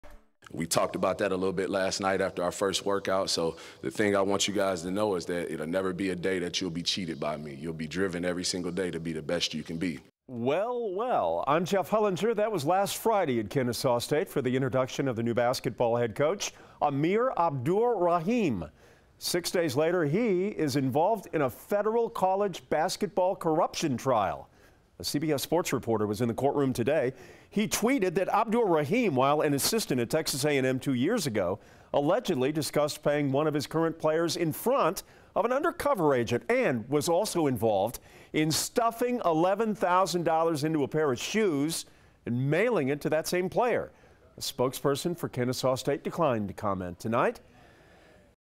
Report: Kennesaw State's new hoops coach implicated in possible pay-to-play scandal
Amir Abdur-Rahim has only been on the job at KSU for six days. But allegations date back to his time as an assistant with Texas A&M.